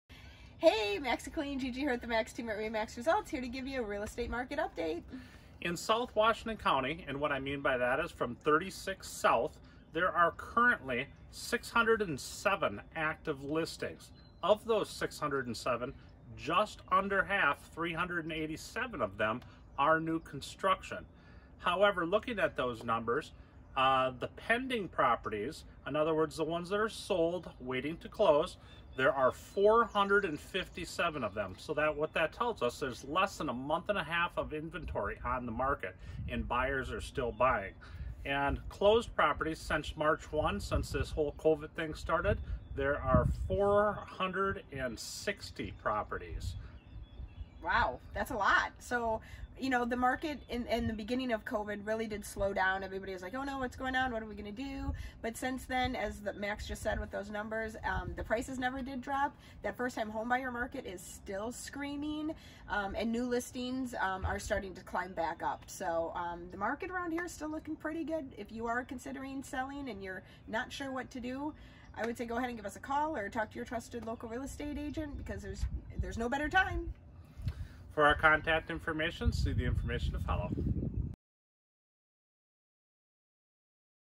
COVID-19 Real Estate Market Update | South Washington County Minnesota Real Estate
What's the real estate market doing during COVID-19? Is it a good time to buy? Sell? Watch our newest market update video to learn everything you need to know in under 90 seconds!